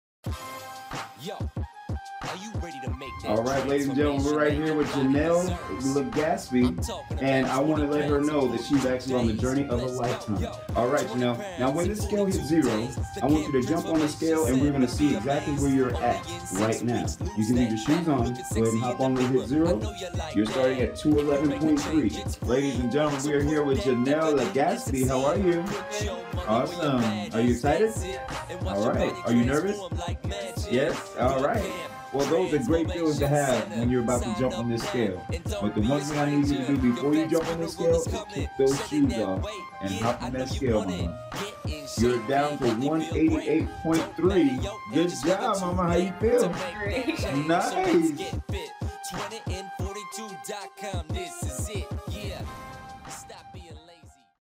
San Fernando Weight Loss Fitness 6 Week Challenge Results - Janelle L.
We will now check the result of Janelle's 6 week weight loss challenge!